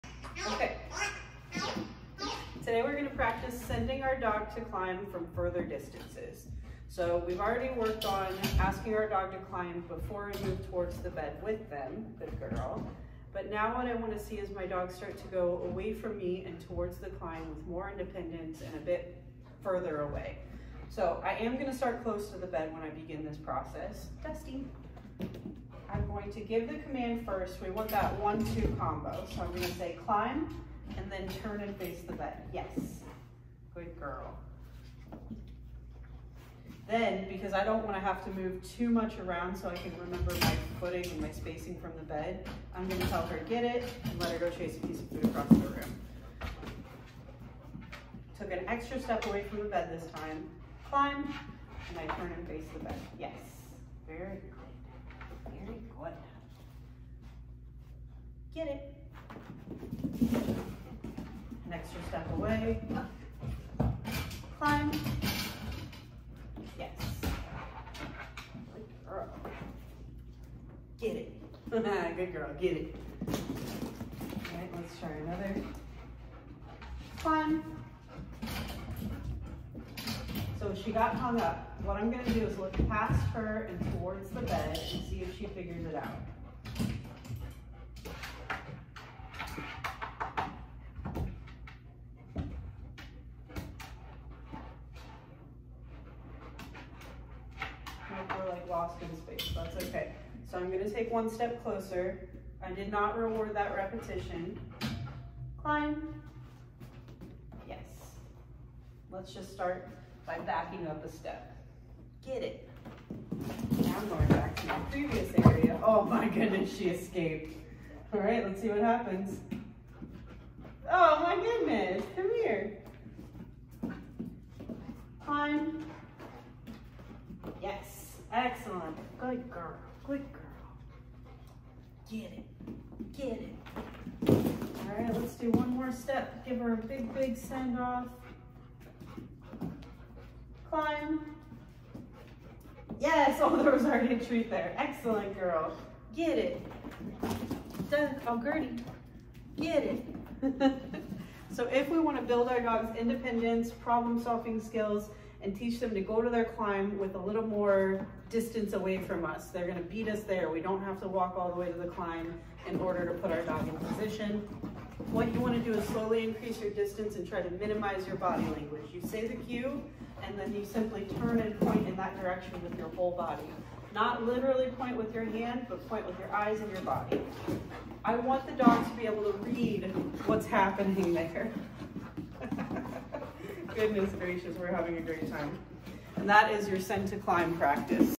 Send to Climb
Try to minimize your body language by asking your dog to climb from increasingly larger distances. If they get stuck, its always worthwhile to stare at the bed and give them a minute to solve it on their own. Restarting is also fair game, they just won’t receive a reward for a failed attempt.